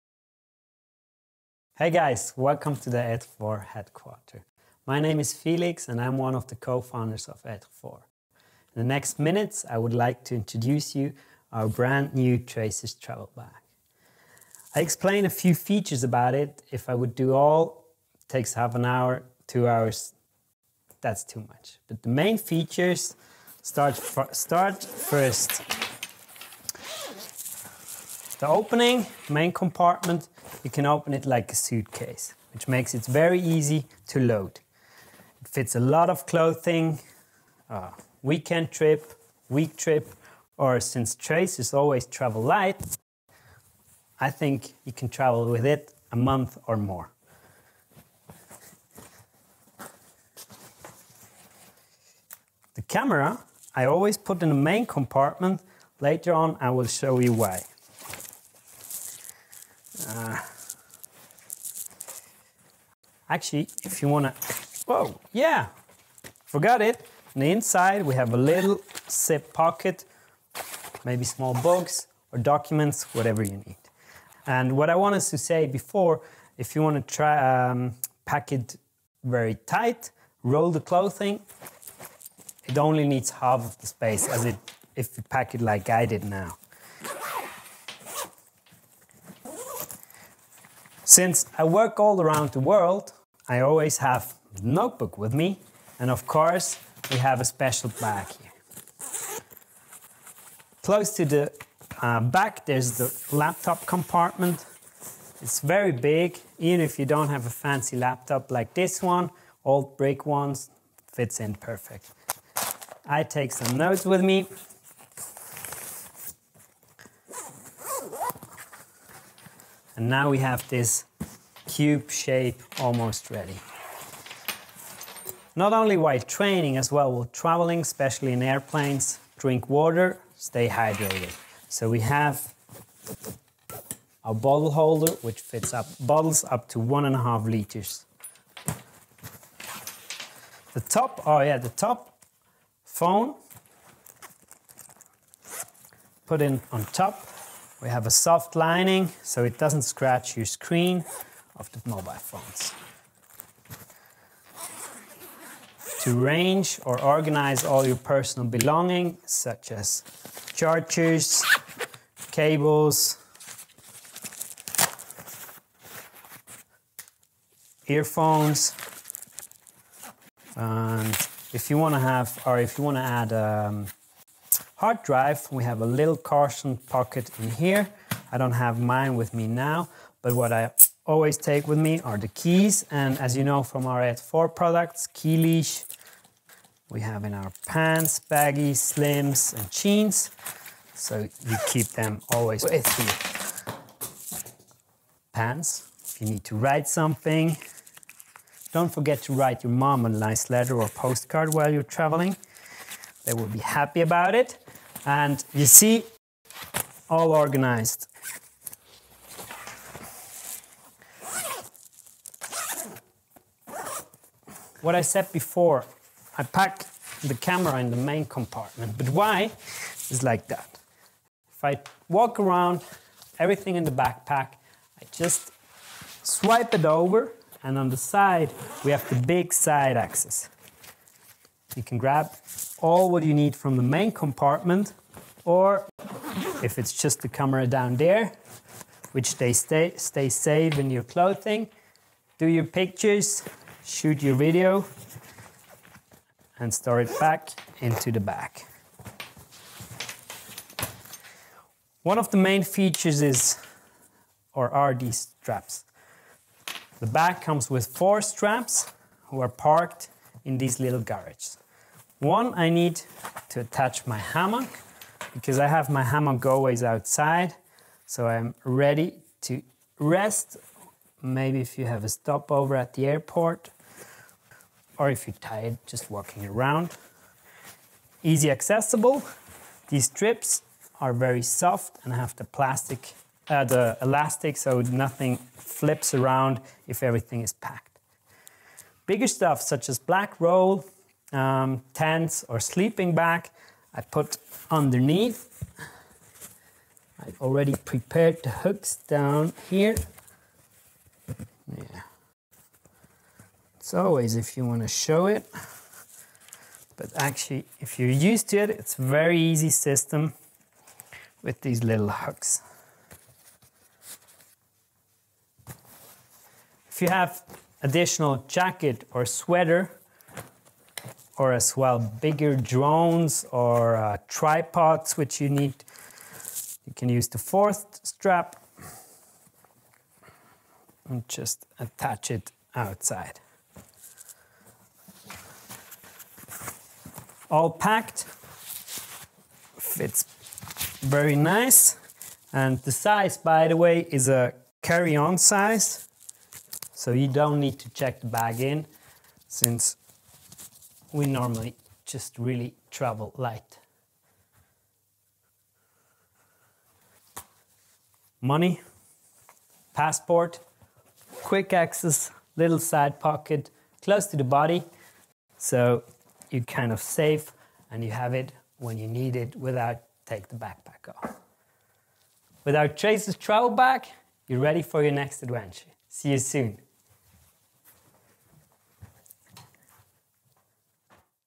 Best Travel Backpack ever? Tracers Travel Bag Preview | ETRE-FORT
We have been waiting eagerly for the new Tracers Travel Bag to arrive. Now it is finally here and ready to ship. We are so happy with this product and also very proud! :)

We have already showed you some of the key features of the bag on our webpage . But that’s far from all of them! Have a look at the video and let Felix Stöckli, ETRE-FORT co-founder, explain a few more. Enjoy!